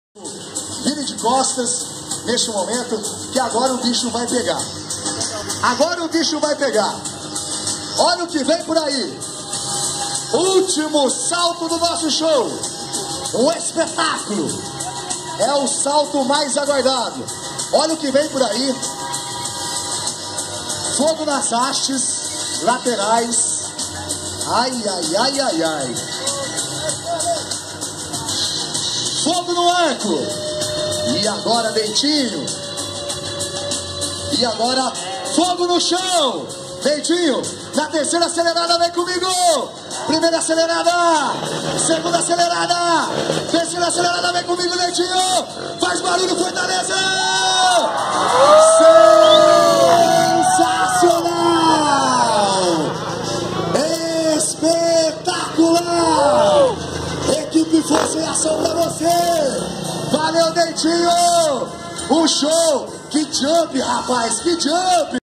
Equipe Força e Ação
apresentação da equipe força e ação no Norh shopping Jóquei!